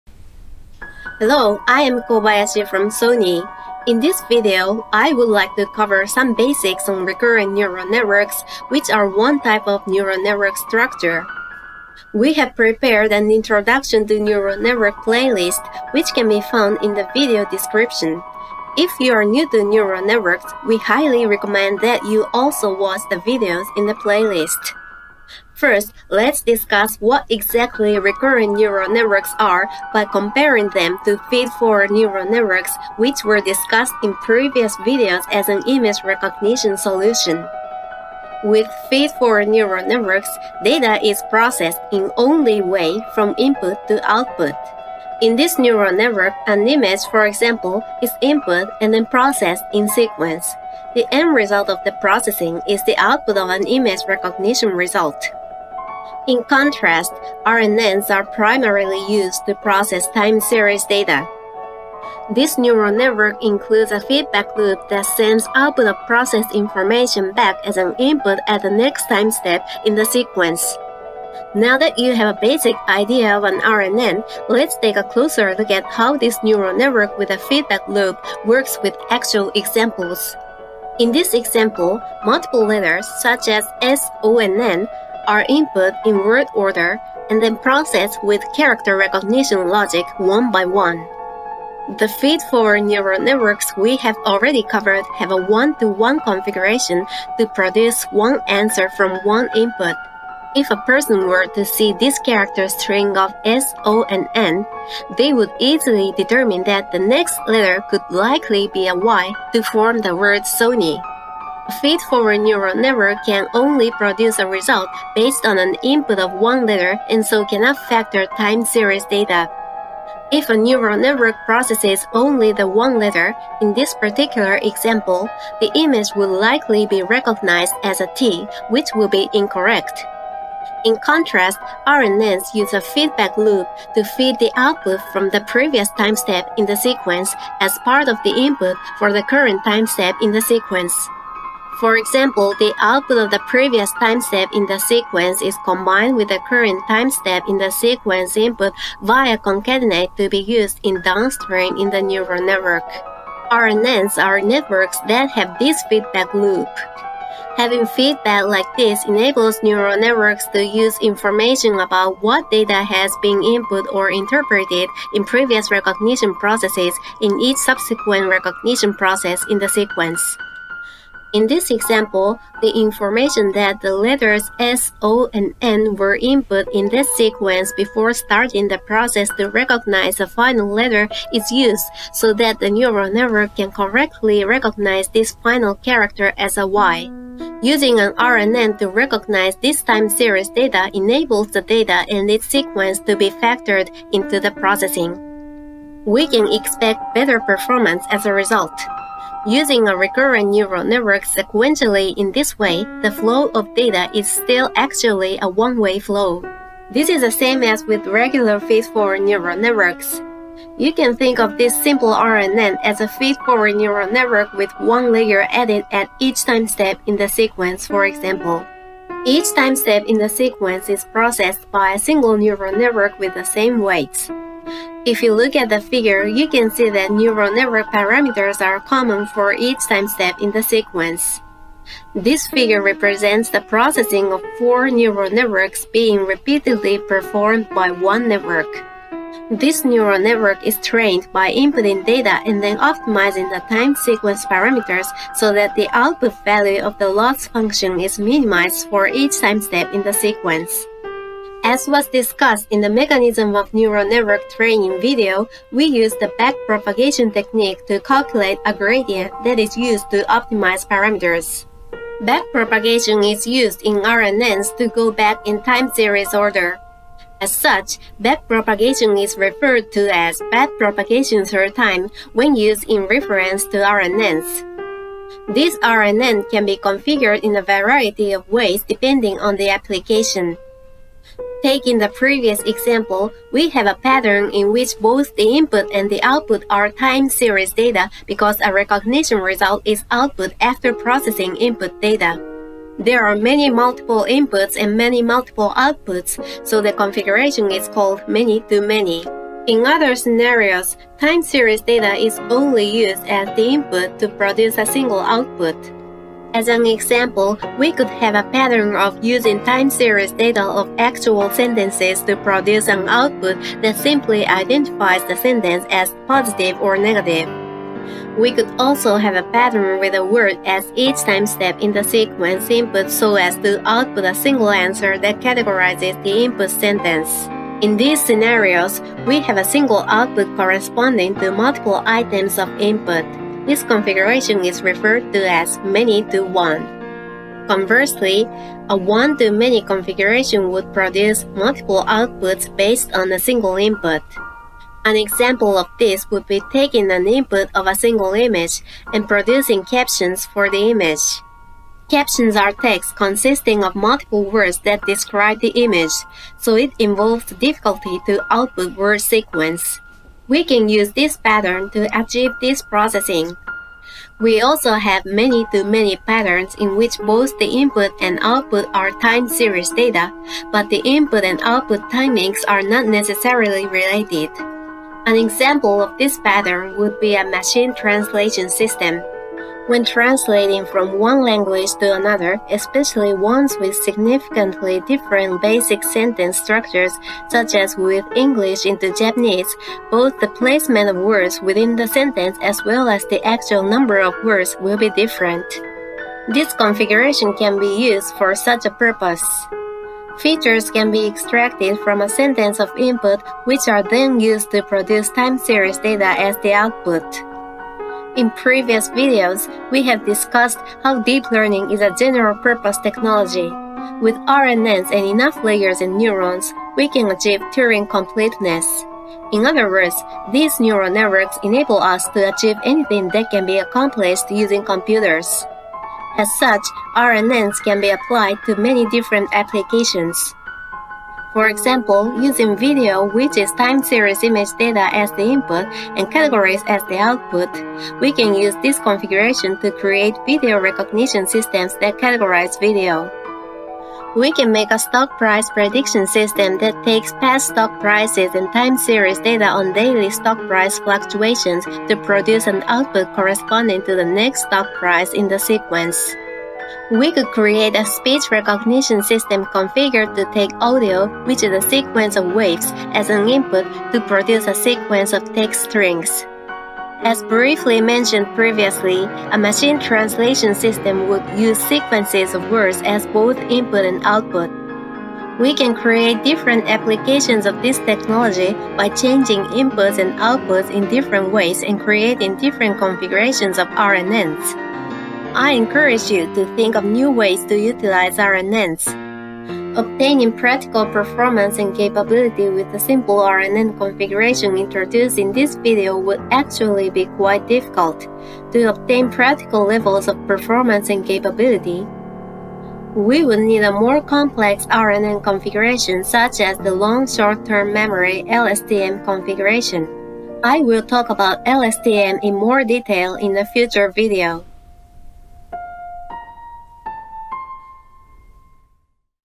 Introduction to Deep Learning : What are Recurrent Neural Networks(RNN)?
This video explains the basics of Recurrent Neural Networks (RNN).

**NOTE**
This contents has been translated from Japanese into English.